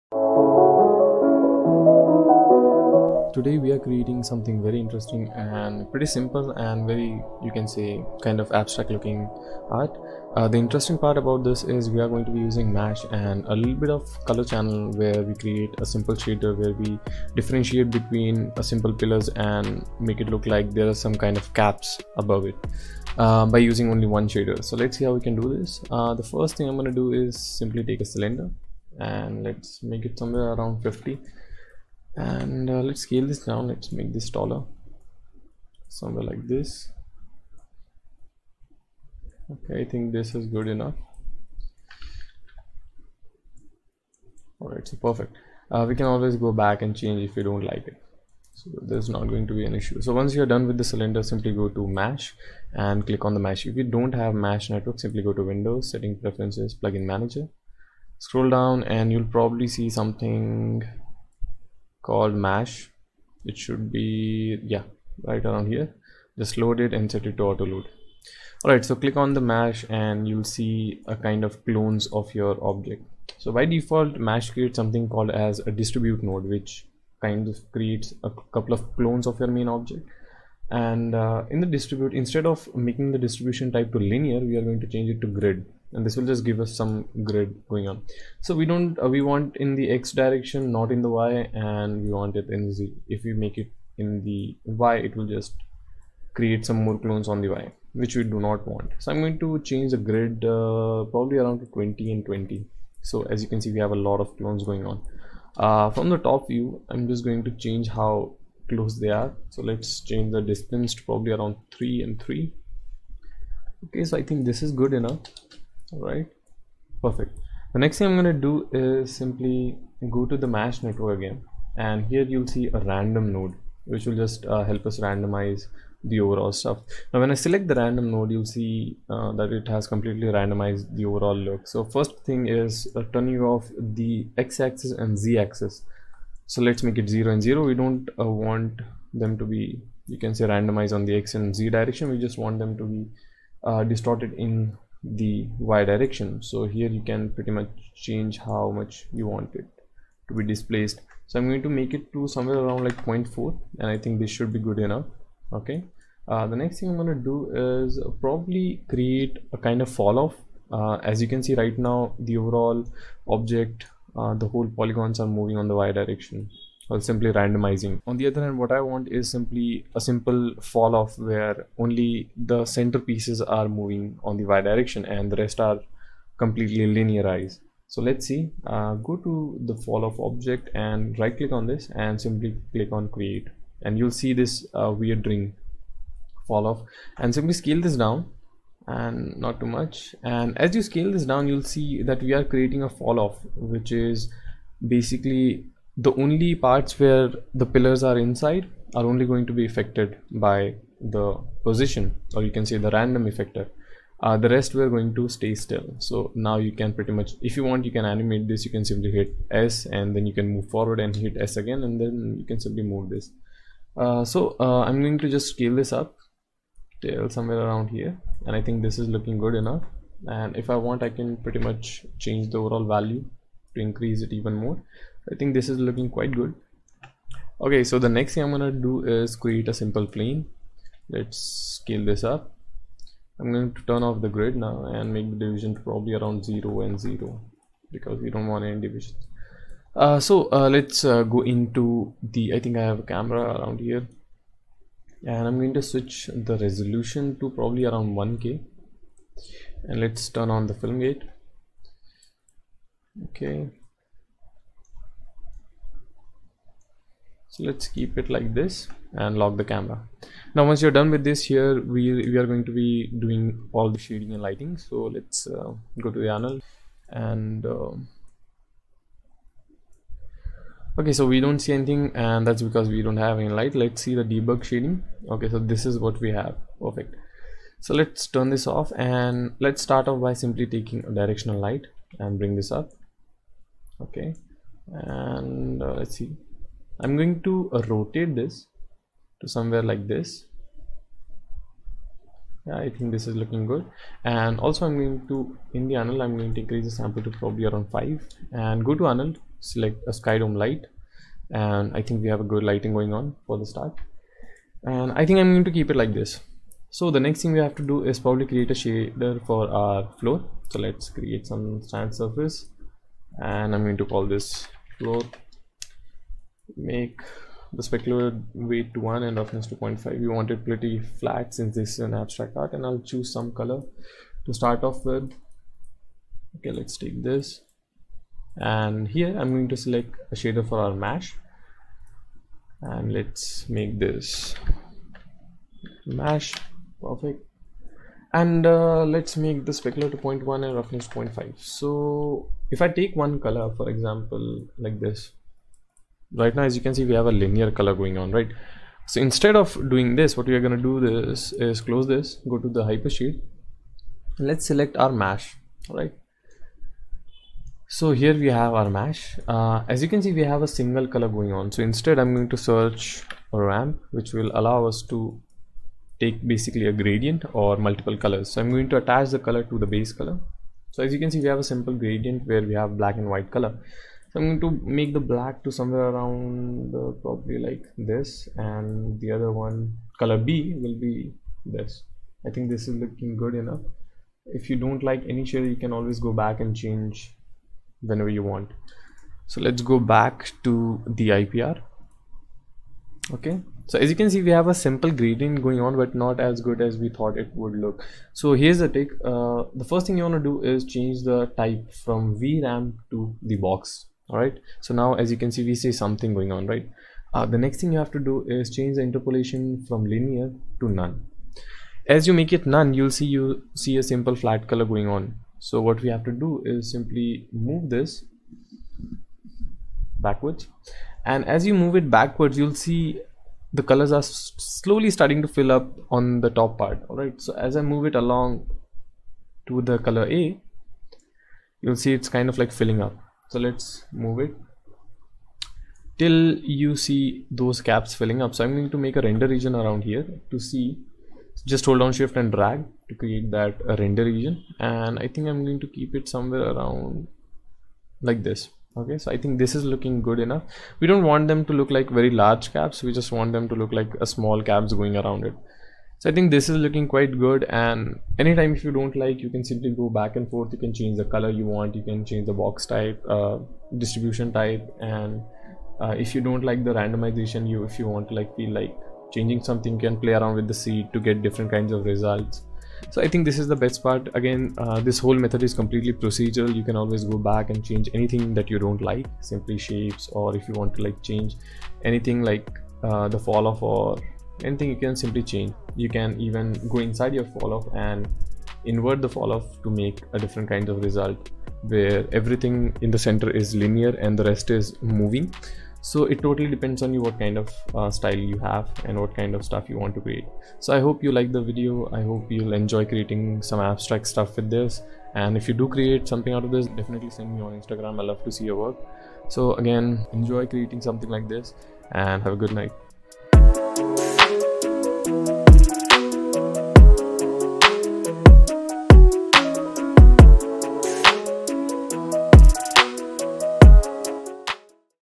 Morphed Pillars Abstract In Maya and Arnold Renderer - Tutorial
Today we are going to take a look at how to create Morphed Pillars Abstract Art in Maya. We're going to be using Mash to create an interesting Abstract Pillar Look.

Behance - amarshindebiz
Vimeo - Ax_Reimagine